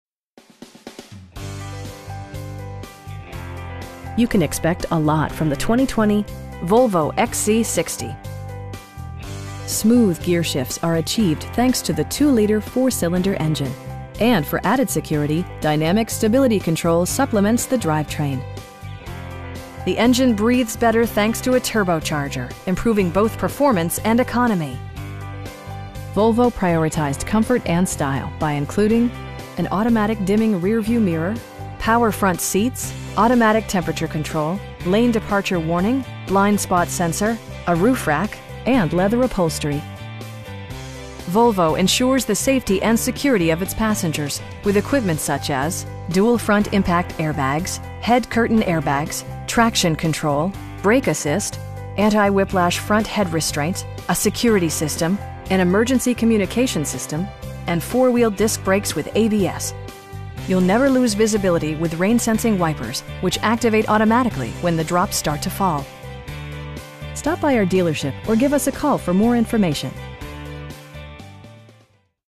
2020 Volvo XC60 T5 Momentum in Charleston, SC 29407
You can expect a lot from the 2020 Volvo XC60. Smooth gearshifts are achieved thanks to the 2 liter 4 cylinder engine, and for added security, dynamic Stability Control supplements the drivetrain. The engine breathes better thanks to a turbocharger, improving both performance and economy. Volvo prioritized comfort and style by including: an automatic dimming rear-view mirror, power front seats, automatic temperature control, lane departure warning, blind spot sensor, a roof rack, and leather upholstery. Volvo ensures the safety and security of its passengers with equipment such as: dual front impact airbags, head curtain airbags, traction control, brake assist, anti-whiplash front head restraints, a security system, an emergency communication system, and 4 wheel disc brakes with ABS. You'll never lose visibility with rain sensing wipers, which activate automatically when the drops start to fall. We pride ourselves in consistently exceeding our customer's expectations. Stop by our dealership or give us a call for more information.